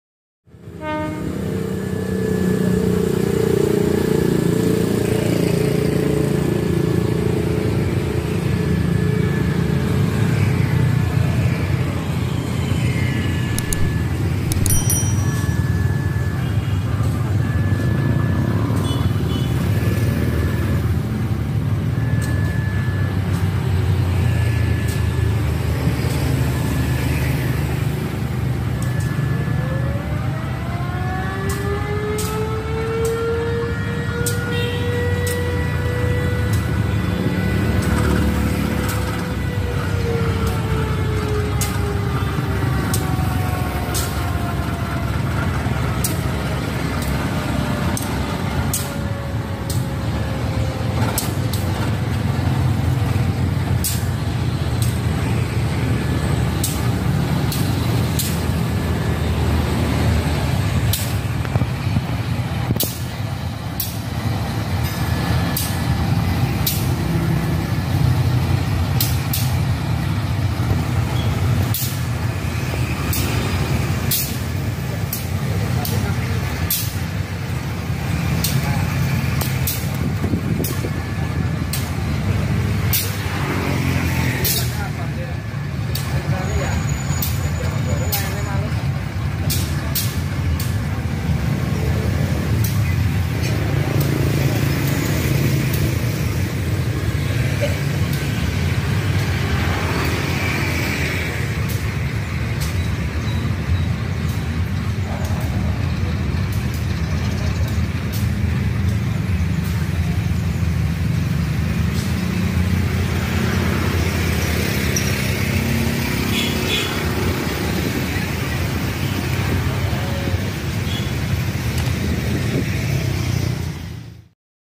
Langsiran Lokomotif Livery Vintage di Balaiyasa Tegal
Perlintasan Kereta Api  di Stasiun Tegal atau Balaiyasa melintas langsiran lokomotif Livery Vintage CC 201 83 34 dengan menarik Rangkaian 1 Gerbong Ballast/Kricak dan 4 GD